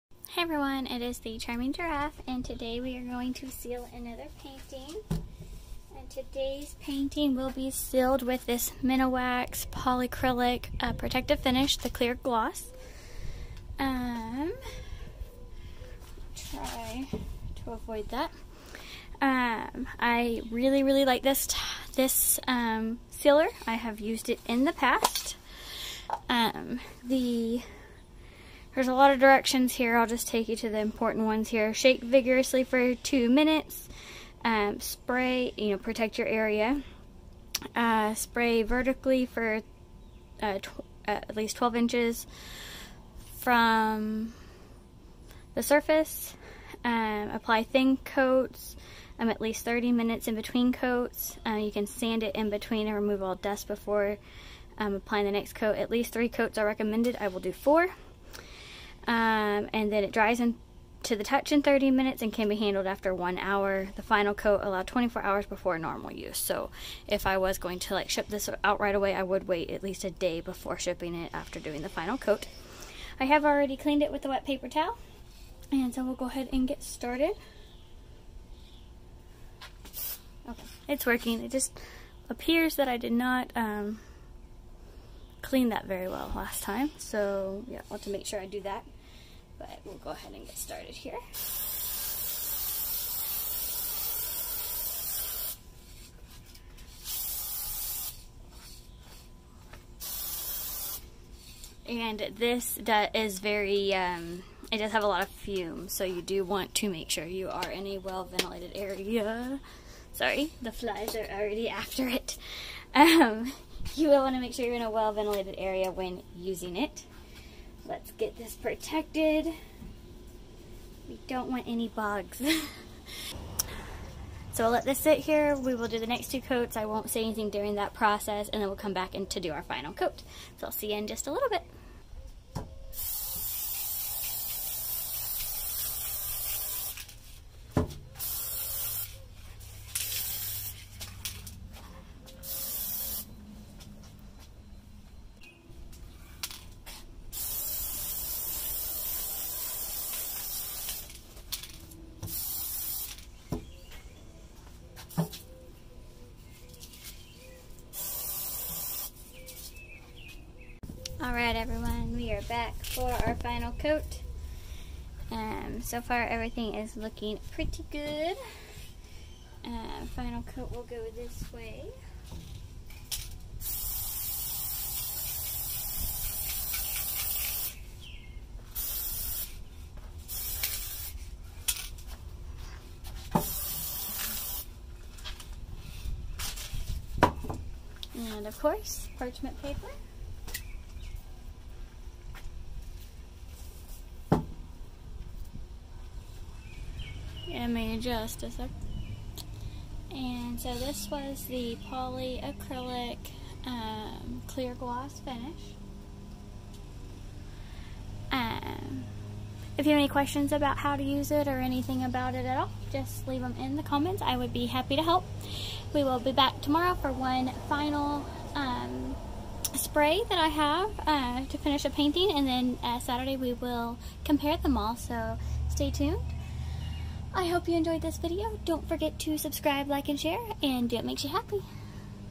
Sealing Acrylic Pour Painting with Minwax Polycrylic Protective Finish Clear Gloss
This week we are testing out different types of acrylic spray sealer/varnish/finish. In this video we use Minwax Polycrylic Protective Finish Clear Gloss to seal an acrylic pour painting! This used to be my favorite one but we'll have to wait and see how they all compare at the end of the week!

Materials used:
-Acrylic Pour Painting
-Minwax Polycrylic Protective Finish Clear Gloss
-Ventilated area
-Food tent
-Wet paper towel

***This video was created for adults***